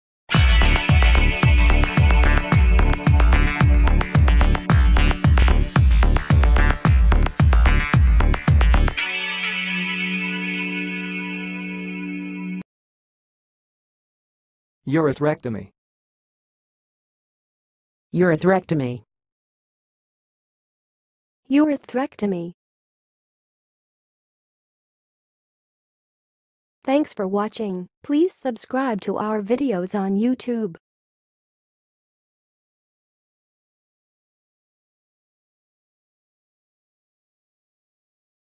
HOW TO SAY URETHRECTOMY?
URETHRECTOMY - All About Urethrectomy: How to Say, Definition, and Use It in 2023

URETHRECTOMY Meaning And Definition:
Urethrectomy is a surgical procedure that involves the removal of all or part of the urethra, which is the tube that carries urine from the bladder to the outside of the body.
It is usually performed to treat conditions such as urethral cancer, urethral strictures, or to address complications from previous surgeries.
URETHRECTOMY Pronunciation:
To pronounce 'urethrectomy', you can break it down into syllables as 'yoo-rihth-REK-tuh-mee'.
Another alternative pronunciation is 'yoo-rih-THREK-tuh-mee'.